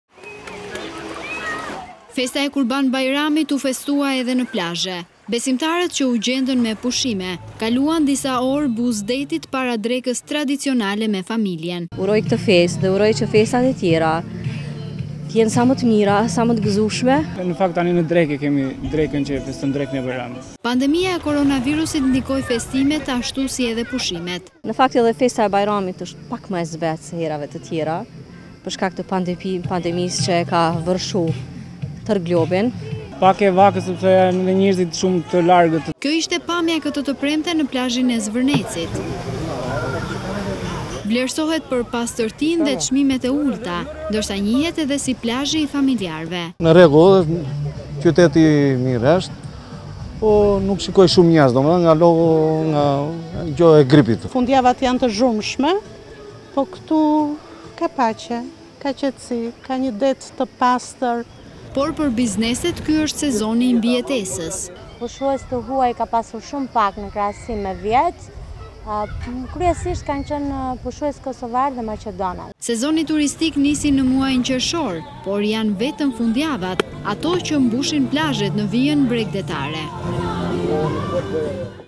Ora News - Vlorë, Kurban Bajrami në plazh, festa fetare kremtohet edhe buzë detit
Prej pak vitesh, tradita ka ndryshuar dhe festat fetare nuk i lënë më bosh plazhet.  Pas riteve fetare, të shumtë ishin ata që iu drejtuan plazhit! Por këtë vit pandemia e koronavirusit i ka ndikuar edhe festimet fetare dhe vete pushimet.

Festa e kurban Bajramit u festua edhe në plazhe.

Besimtarët që u gjenden me pushime, kaluan disa orë buzë detit para drekës tradicionale me familjen.

“Uroj këtë festë dhe uroj që festat e tjera të jenë sa më të mira.”

“Tani në drekë e festojmë ne.”

Pandemia e koronavirusit ndikoi festimet ashtu si edhe pushimet.Follow TV Ora dhe RTV Ora News